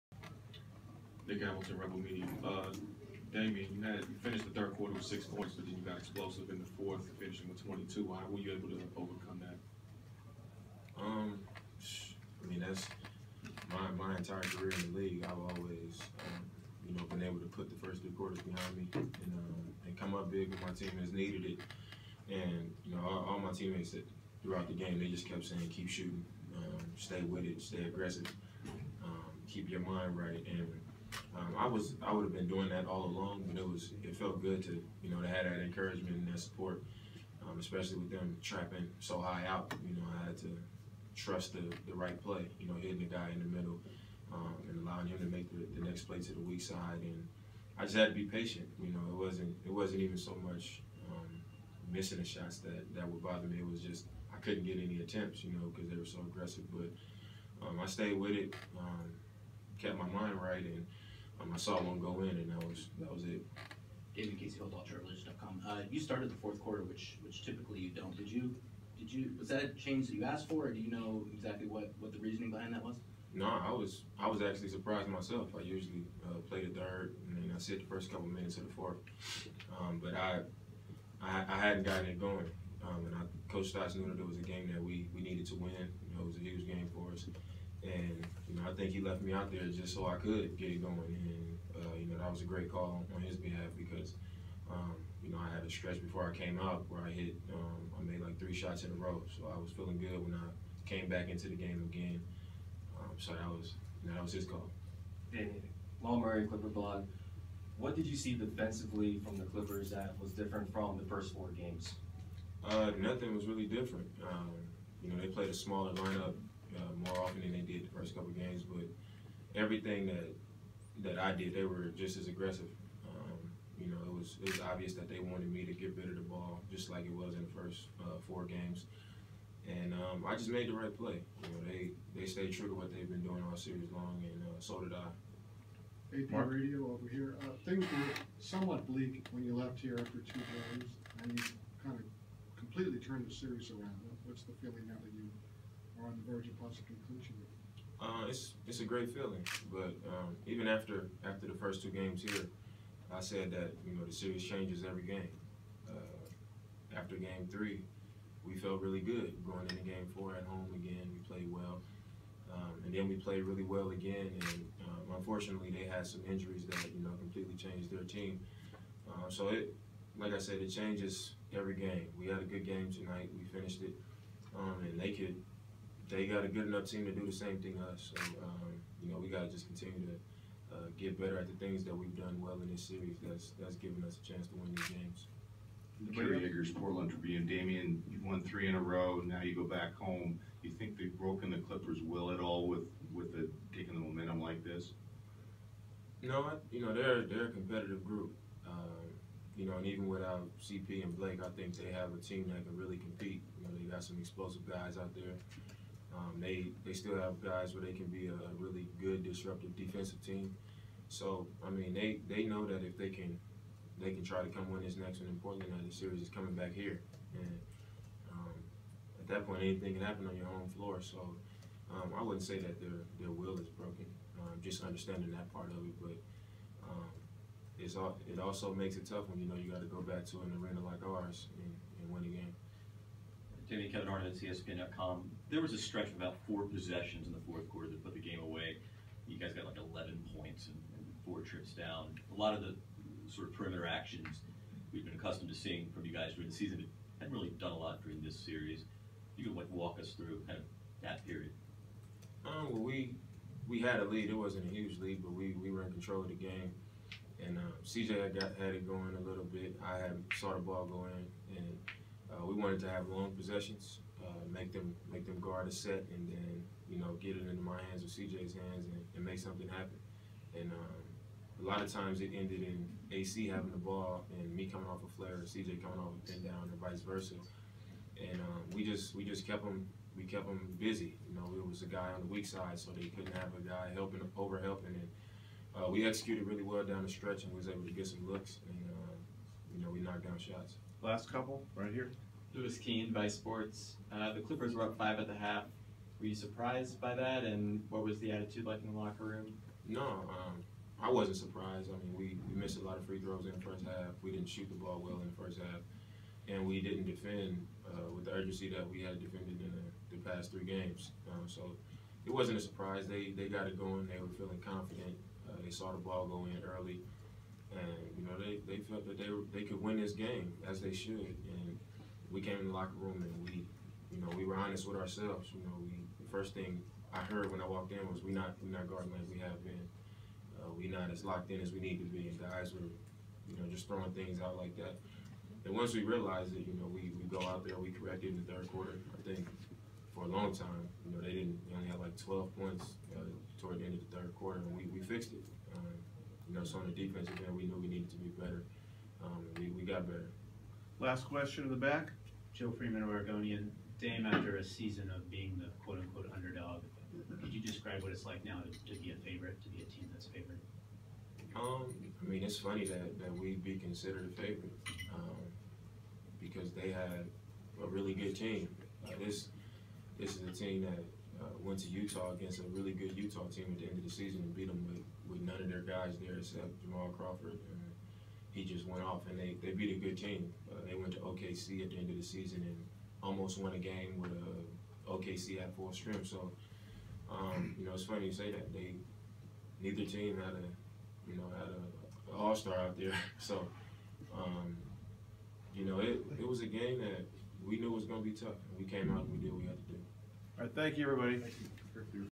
2016 Playoffs - Rd 1 Game 5 - Clippers vs Trailblazers
Post Game Press Conference With Damian Lillard